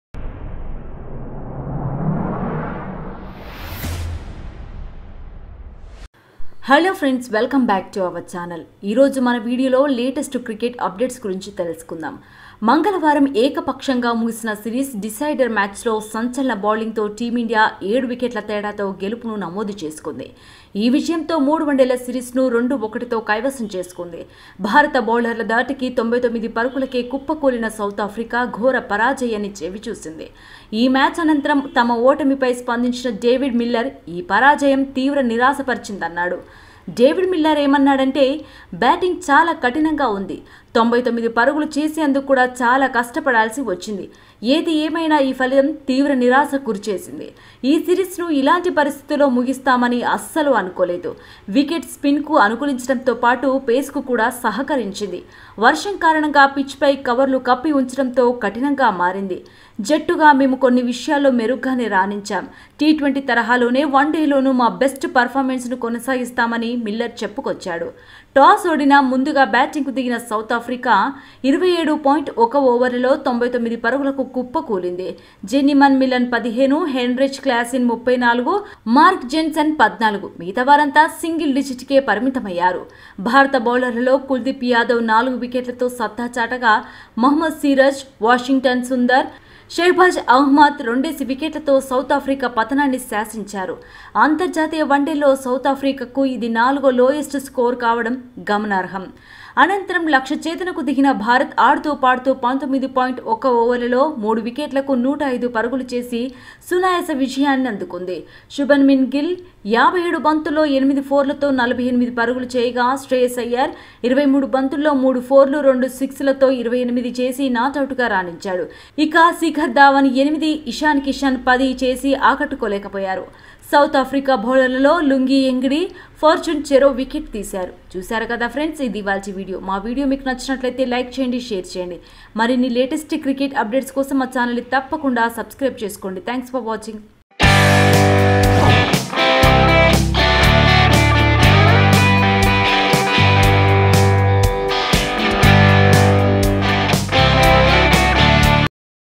David Miller Comments On South Africa Loss Against Team India In 3rd ODI|IND vs SA 3rd ODI Updates
Watch: David Miller Comments On South Africa Loss Against Team India In 3rd ODI|IND vs SA 3rd ODI Updates